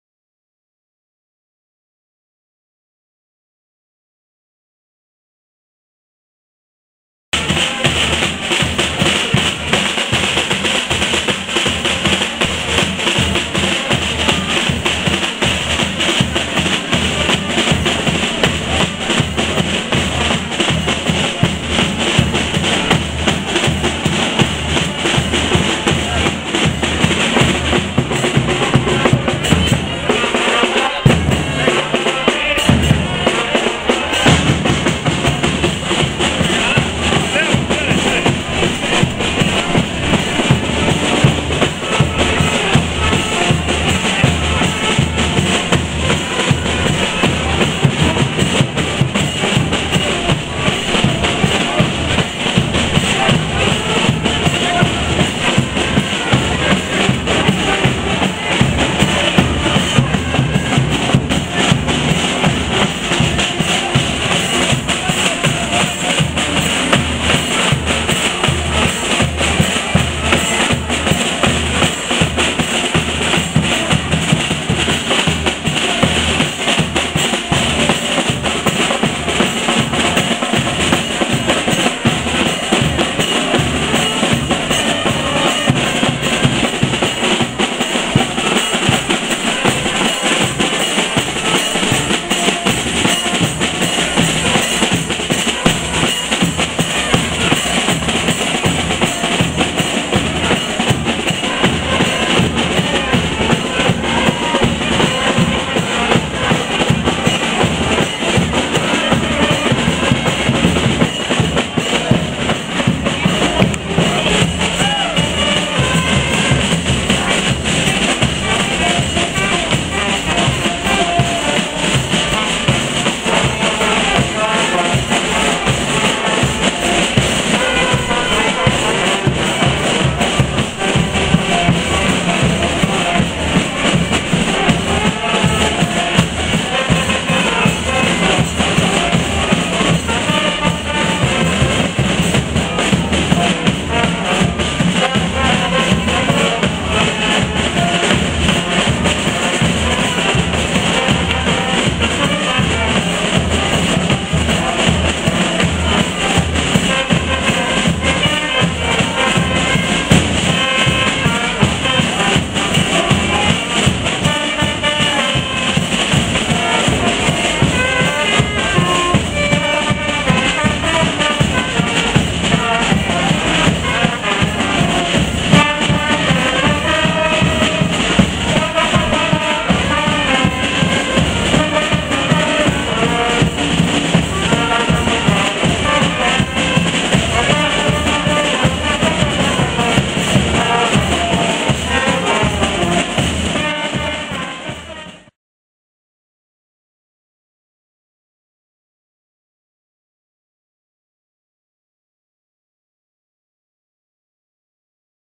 TVNOVAERA - CARNAVAL 2013 NOVA ERA RUA GOV.VALADARES
Vídeo produzido por tvnovaera, na Rua Gov.Valadares, no Carnaval de 2013.